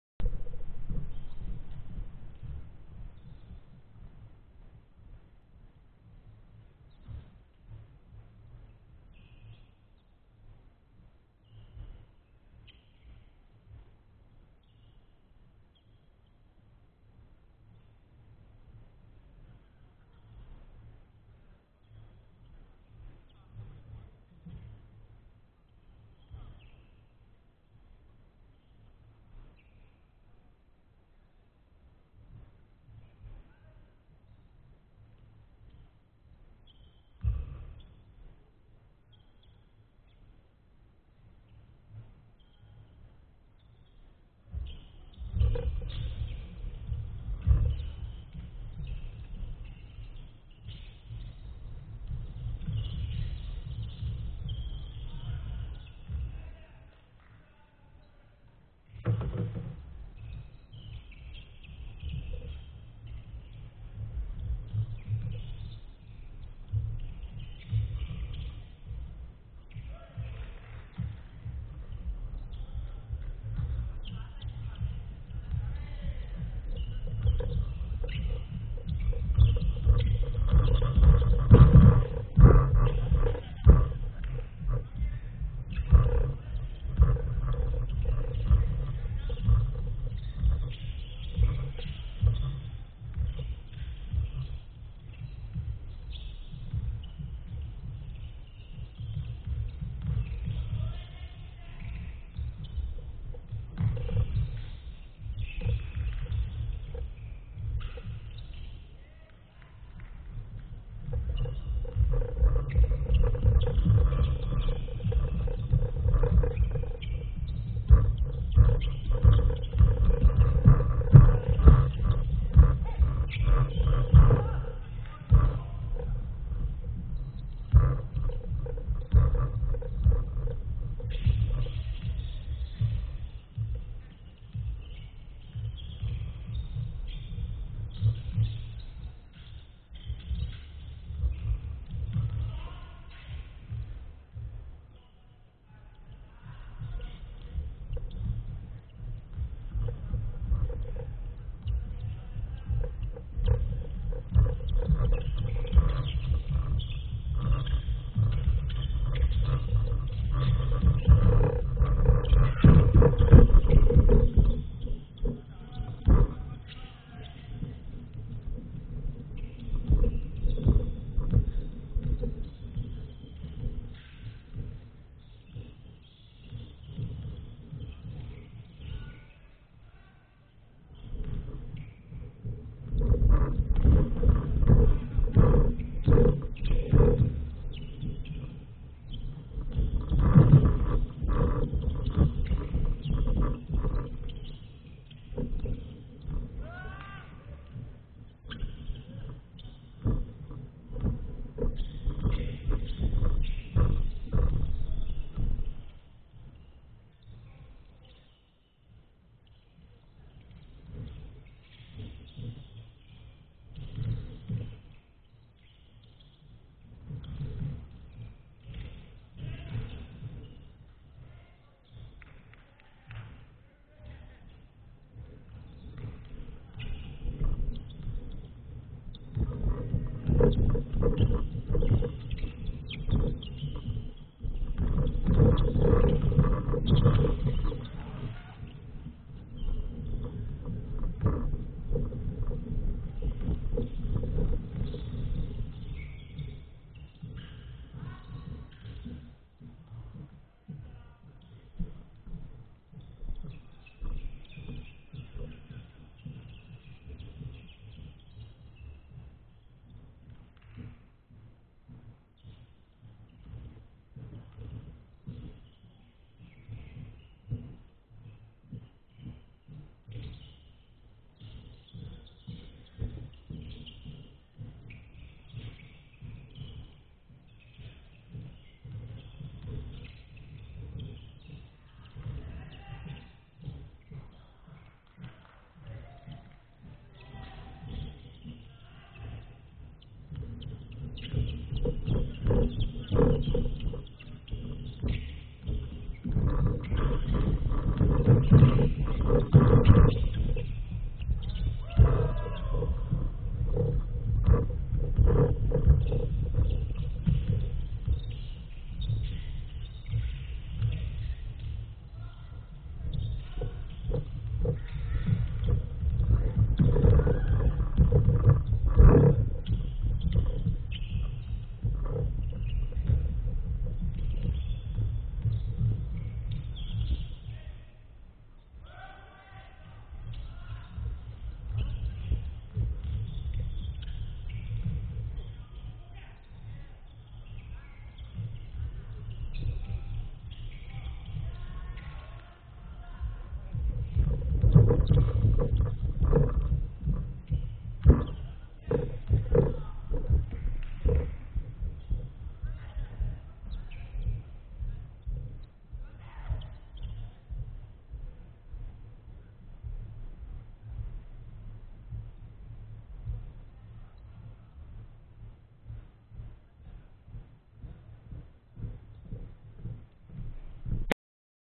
【20220829】Liberewsリベルーズ 練習 風景_5on5_12
福岡県で活動している Liberews(リベルーズ)の練習動画です。
毎週月曜日に20時から練習してます！